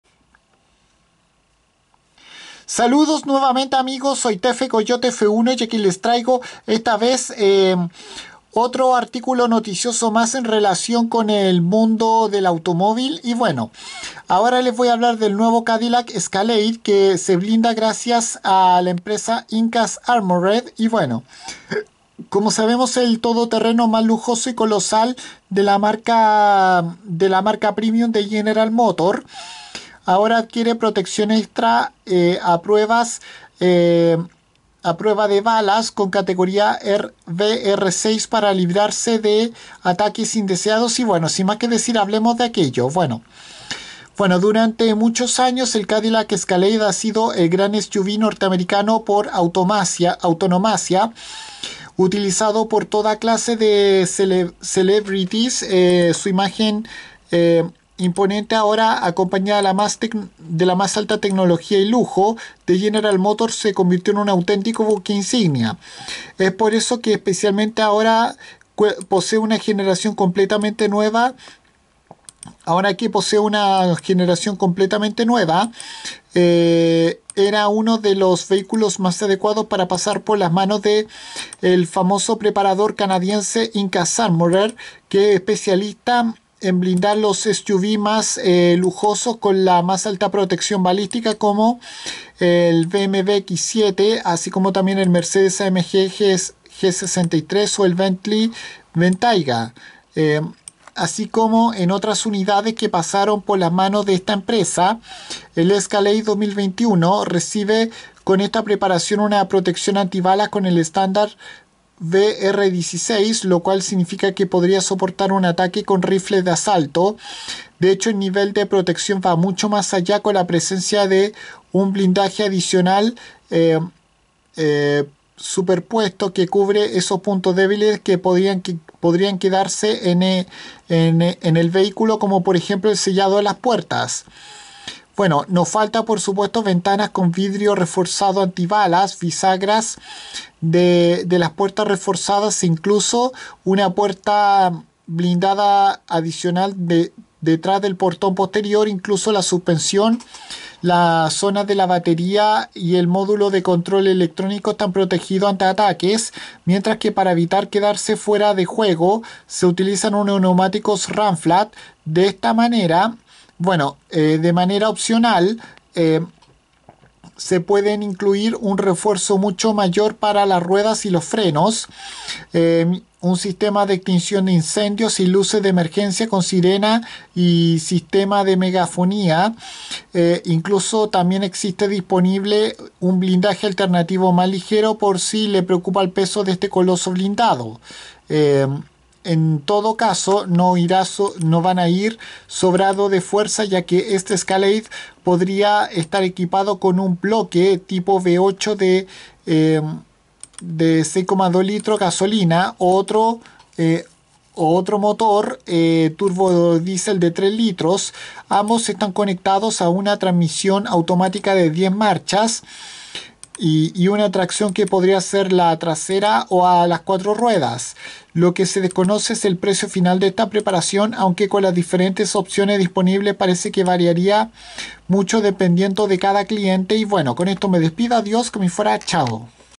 El nuevo Cadillac Escalade se refuerza con blindaje de Inkas Armored
Aquí les cuento sobre como la nueva versión del lujoso todoterreno de la marca automotriz premium de General Motors adquiere protección antibalas adicional de categoría BR6 para evitar ataques no deseados